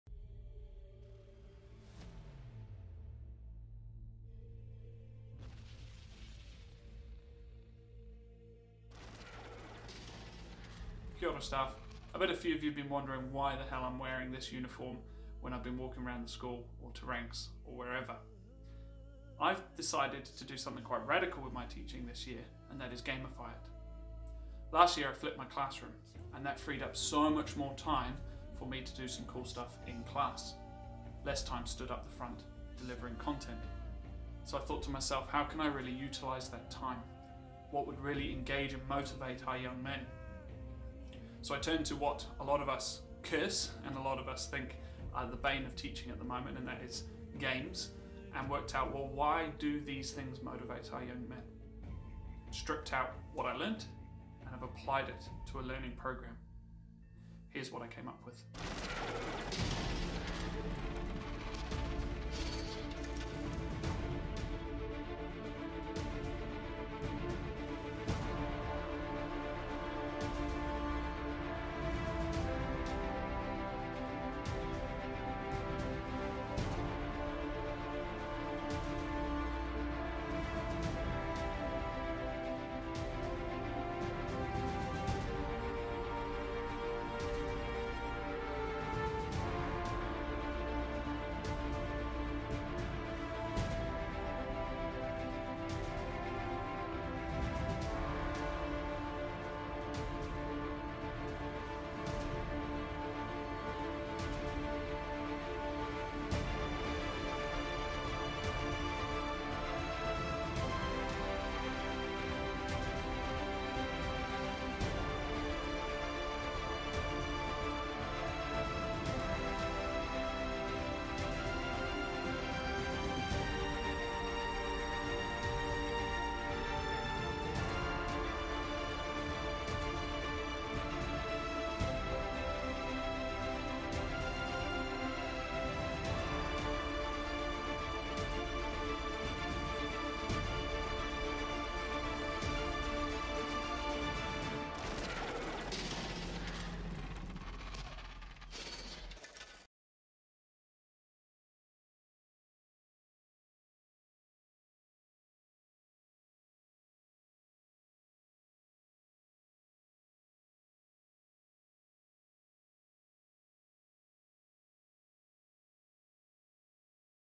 Gamification Teaser SPC Staff PD
He is a taster of what I will be covering in my part of this afternoons PD session. Make sure you come armed with those 3 Essential questions!